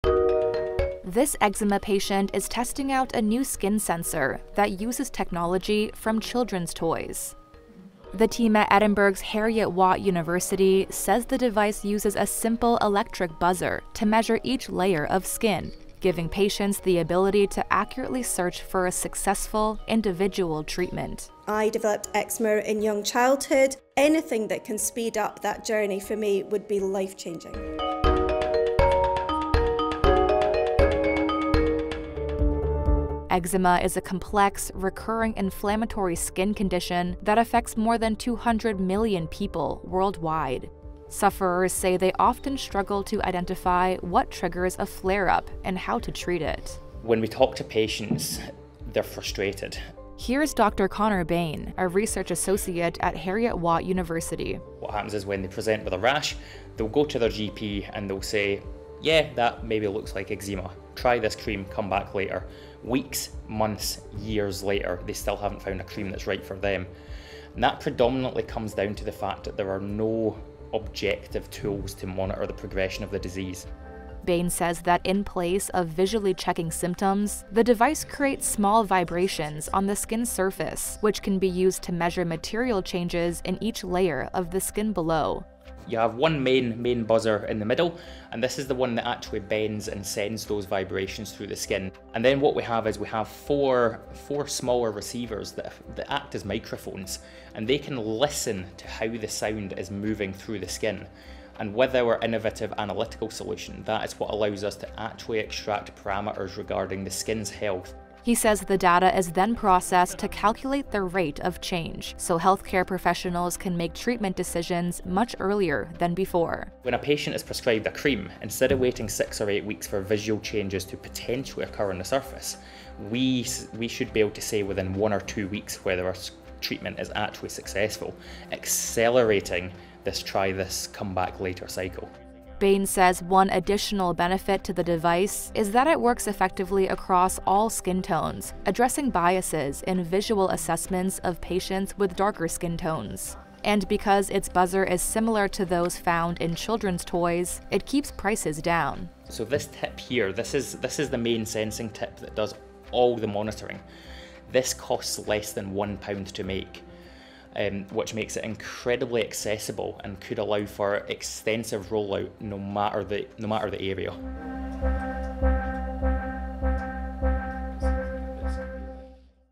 How children's toy tech could change the way eczema is treated | REUTERS
Technology from a child's toy is the basis of a new skin sensor, which could help transform the treatment of eczema and psoriasis.